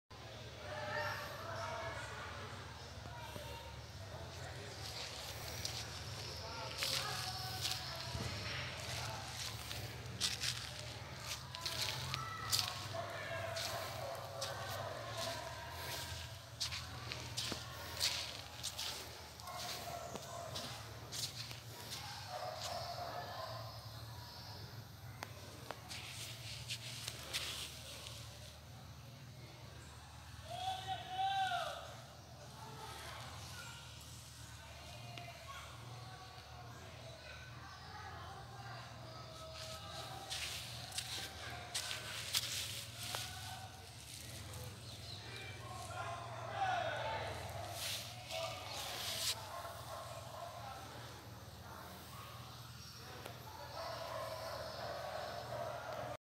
Rio claro caraguatatuba sp
venha conhecer as cachoeiras do rio claro em Caraguatatuba sp